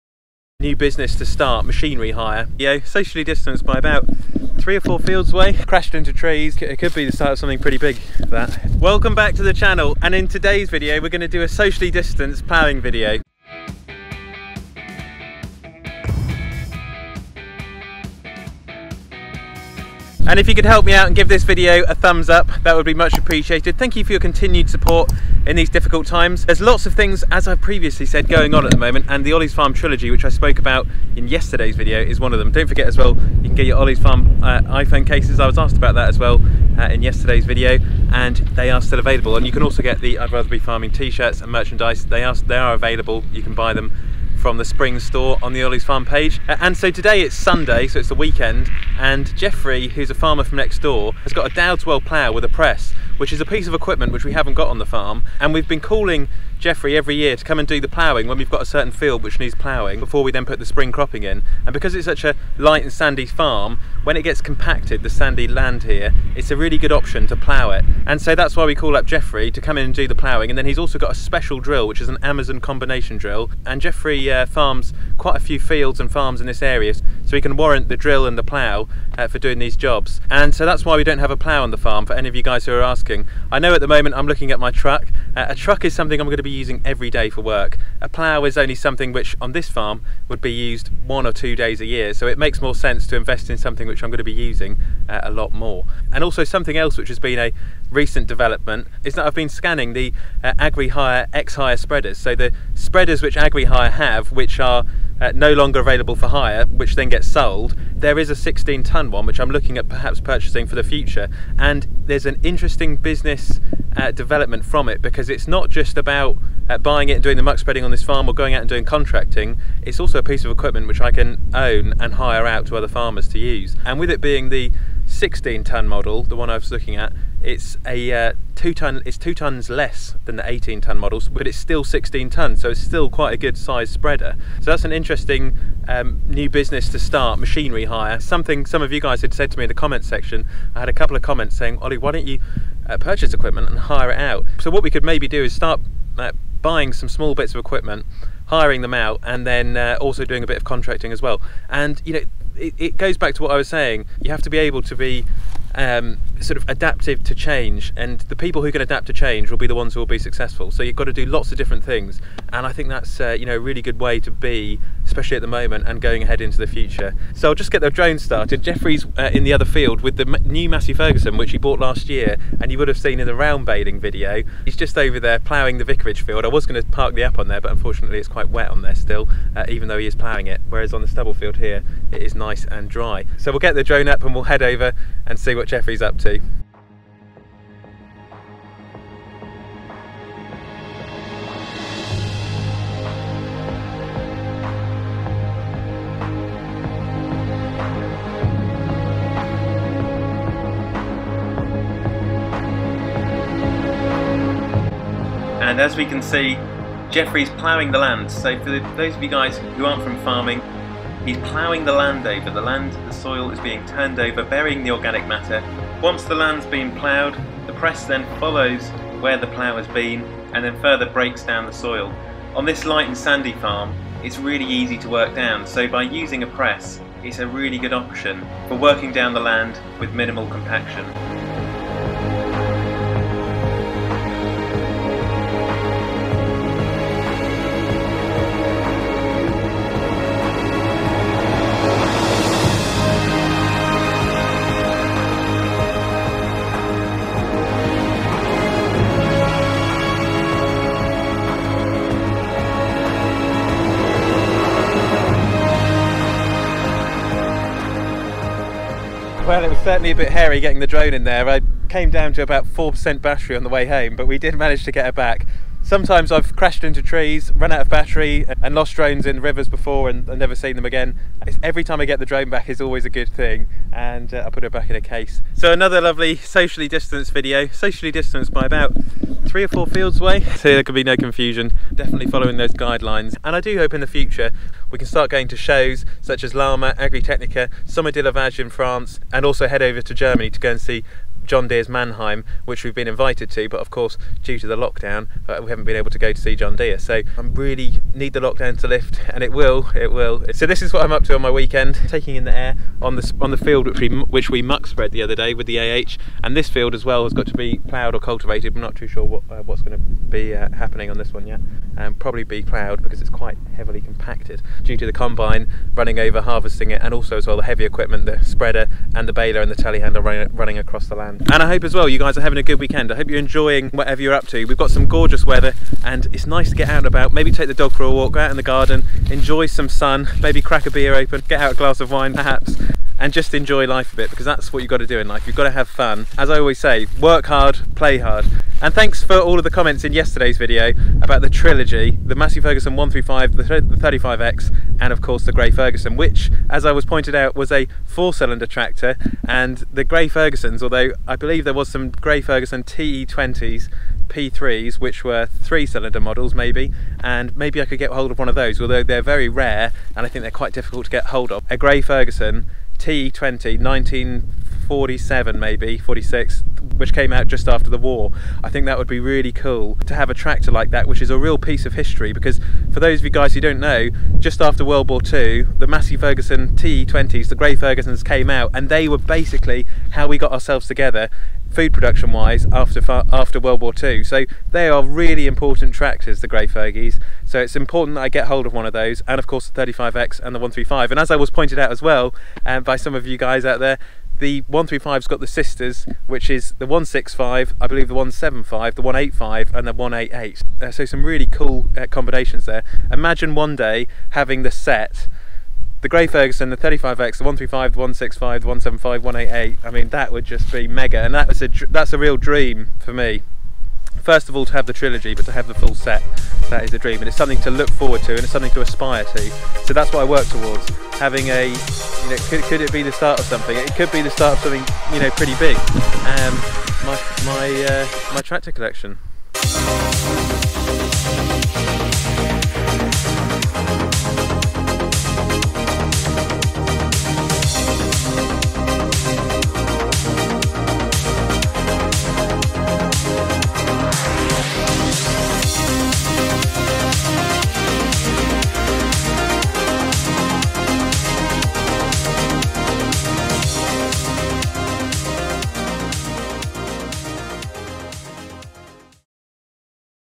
GETTING IT DONE IN ONE PASS PLOUGHING AND PRESSING WITH NEW MASSEY !
In today's video the x potato field we cultivated is being ploughed and pressed by the five furrow dowdeswell plough and press, we plough the land to turn over the soil, which helps to break compaction, bury the trash from the previous crop, and work down the soil using the press behind the plough. On this light and sandy farm it works really well using the press because the light sandy land can be broken down easily by the press, which reduces the need for another pass which in turn reduces further compaction from machinery. We also discuss a new business idea, and talk about the trilogy and the collection... Enjoy today's video!

Follow Olly's Farm On

To see Olly's daily antics

Snapchat Search - Olly's Farm